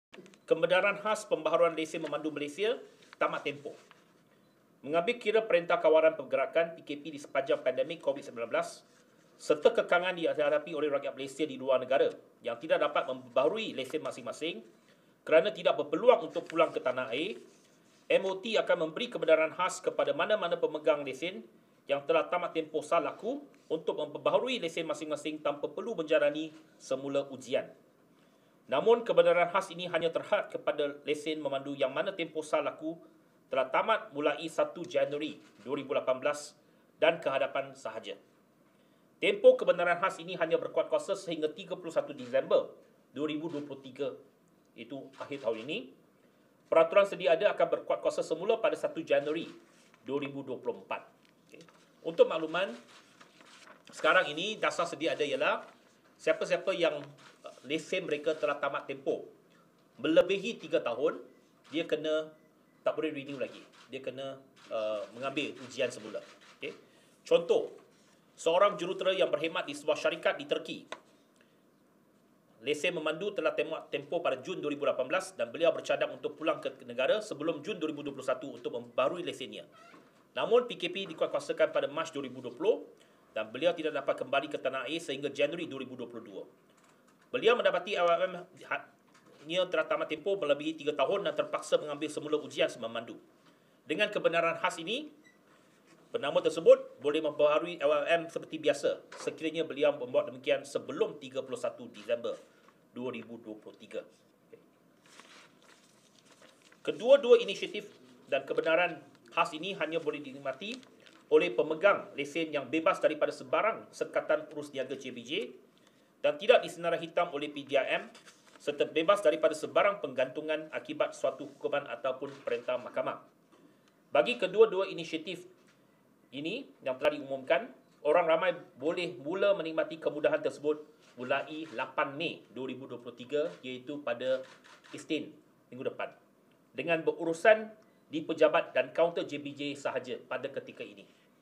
Kebenaran khas perbaharui lesen memandu tamat tempoh selepas 3 tahun
Menteri Pengangkutan, Anthony Loke mengumumkan inisiatif kebenaran khas untuk memperbaharui lesen yang tamat tempoh sah selepas 1 Januari 2018 ke atas.
Katanya, pemegang lesen juga mesti bebas daripada senarai hitam, penggantungan atau sebarang perintah mahkamah.

AWANInewsSUBSCRIBE now! @Astro Awani